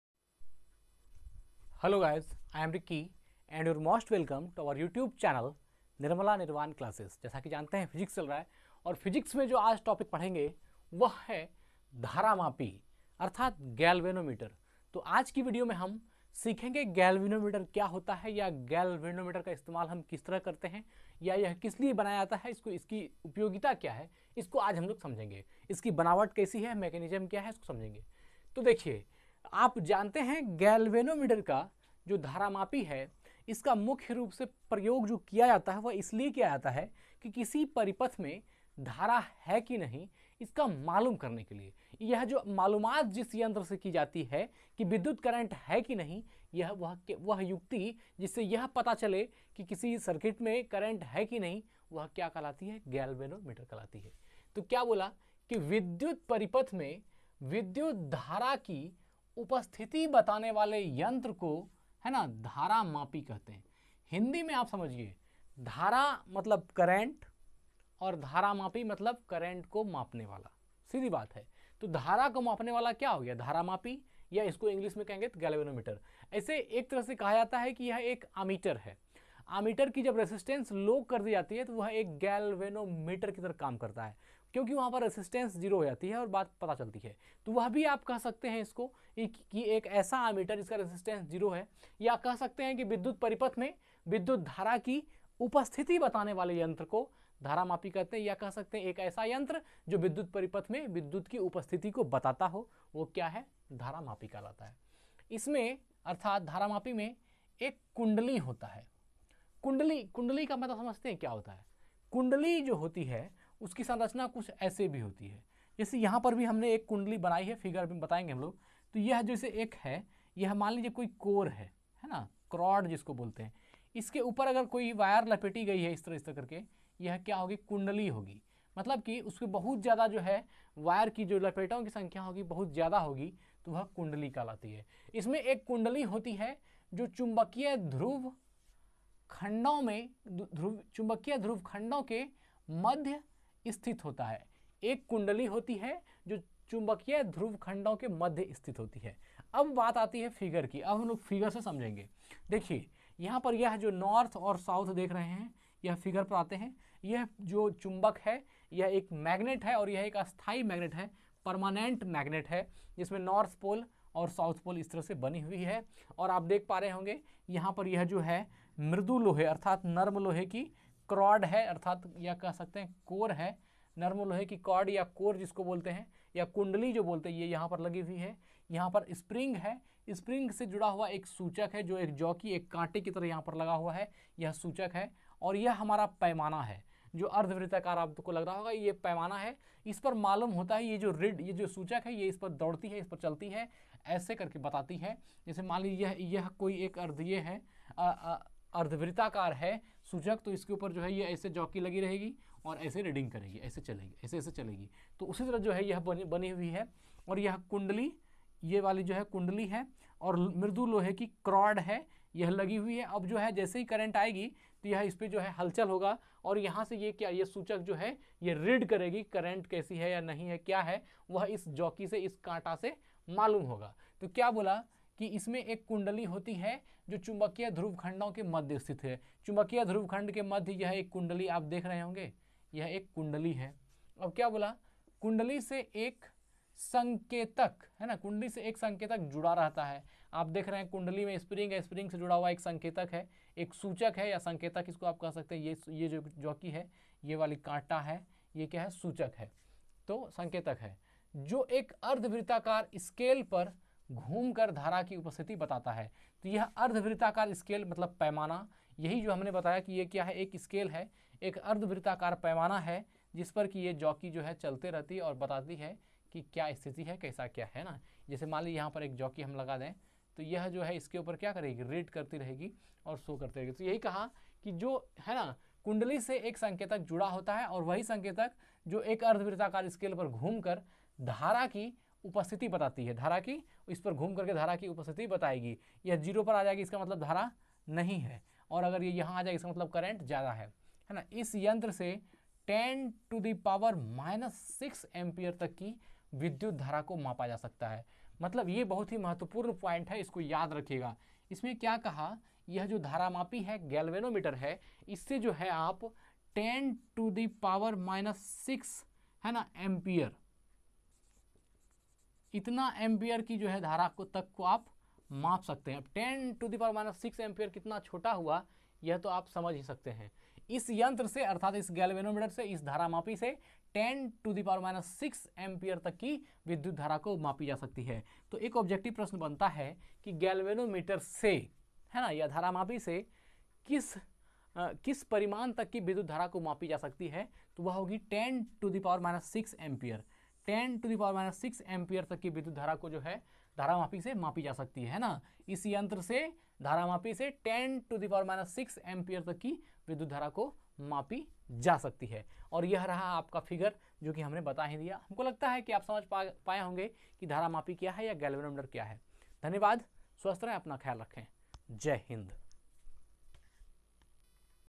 (171)धारामापी(GALVANOMETER)
धारामापी(GALVANOMETER)
 इस वीडियो में धारामापी के बारे में पूर्ण जानकारी दी गई है. इसमें इस बात को समझाने का प्रयास किया गया है कि धारामापी क्या होता है या इसकी क्रिया विधि कैसे हैं. हमें आशा ही नहीं बल्कि पूर्ण विश्वास है कि जो भी छात्र इस वीडियो को देखेंगे वह अच्छी प्रकार समझ पाएंगे कि आखिर धारामापी होता क्या है.